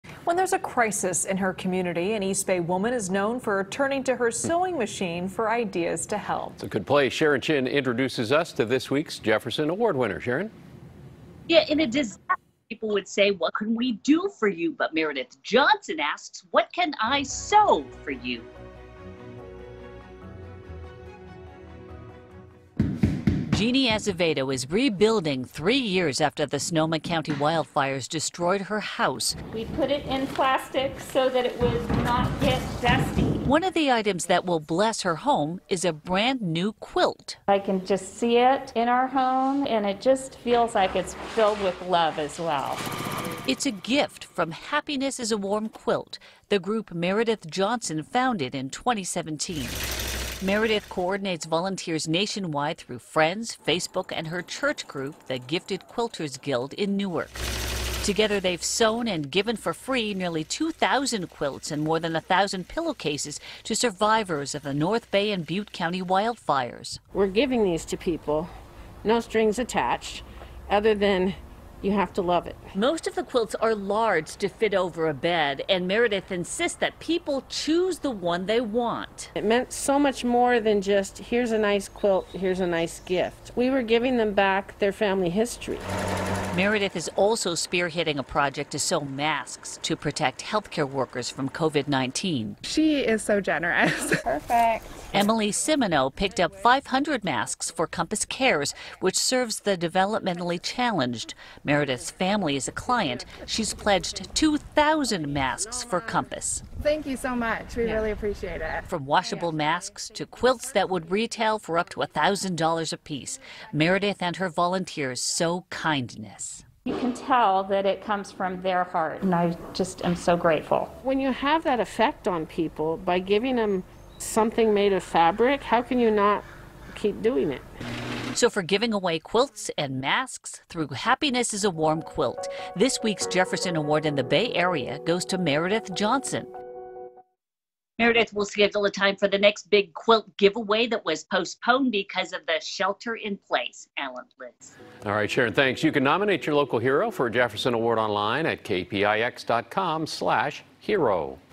East Bay Woman's Non-Profit Creates Quilts To Give Comfort To People In Crisis
When there's a crisis in her community, an East Bay woman is known for turning to her sewing machine for ideas to help. Sharon Chin reports. (6/3/20)